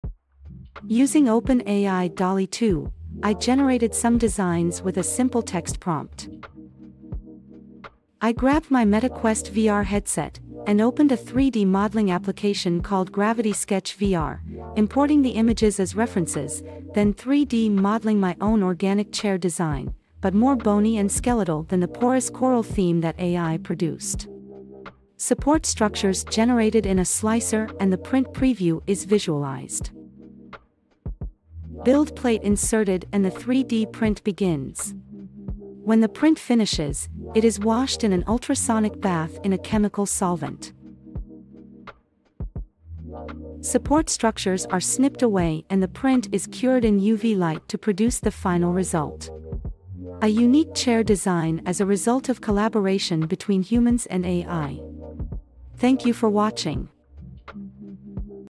I 3D Printed this AI Designed Chair
Using a combination of AI generated images, virtual reality 3d modelling and resin 3D printing to produce a unique chair design.